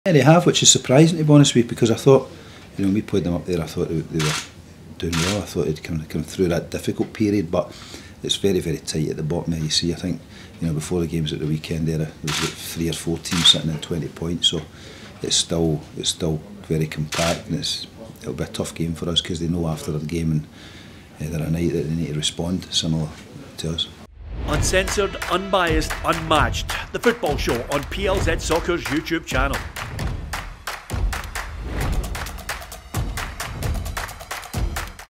Robbie Neilson admits surprise at Dundee United's league position
🗣️ 'They know after their game the other night that they need to respond, similar to us.'

Robbie Neilson admits 'surprise' at Dundee United's league position, as he expects response from Kilmarnock defeat